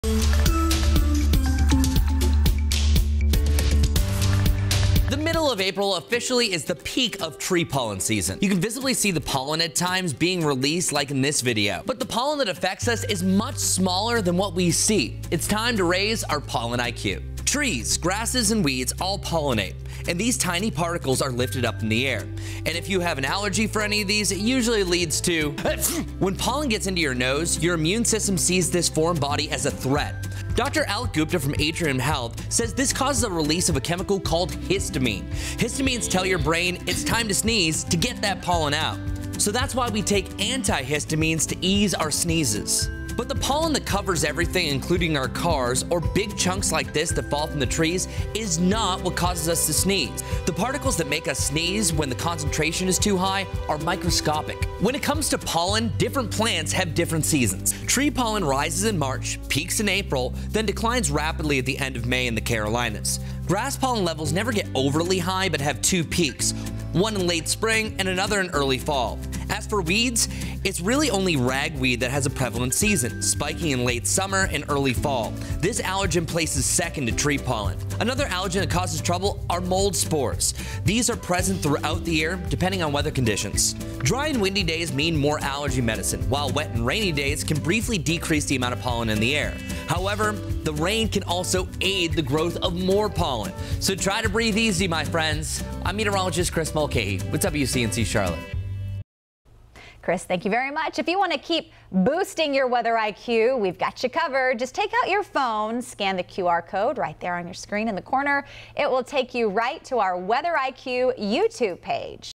Weather IQ: How pollen impacts us in the Carolinas
The middle of April is officially the peak of tree pollen season. You can visibly see the pollen at times.